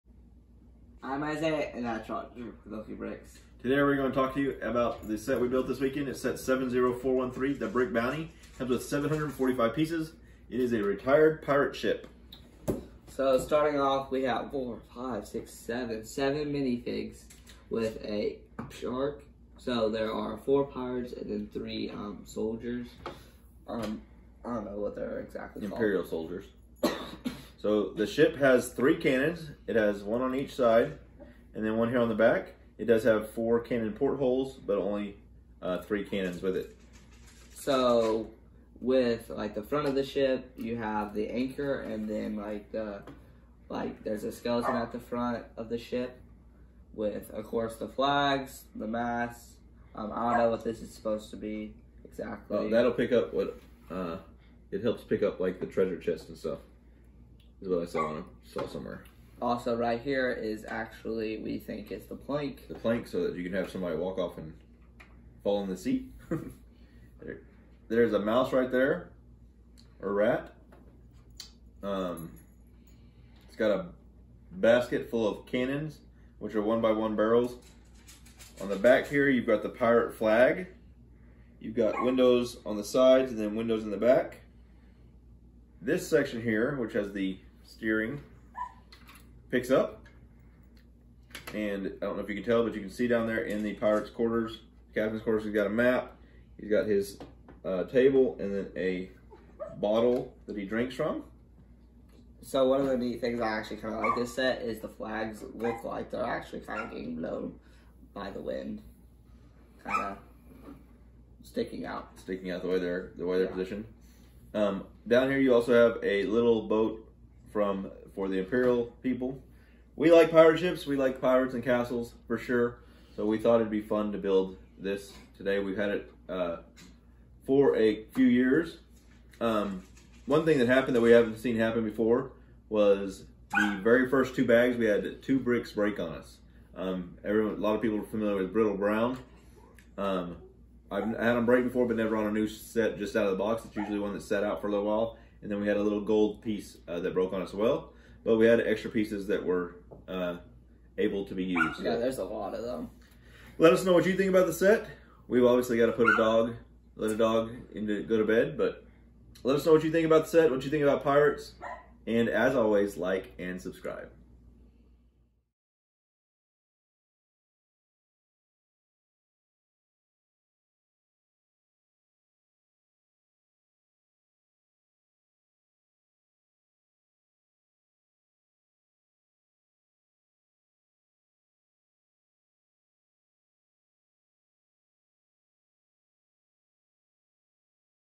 LEGO Brick Bounty Pirate Ship
LEGOPirateShips are so cool! We really enjoyed building this LEGO set the past couple of days. We have several ships and this one will be a great addition.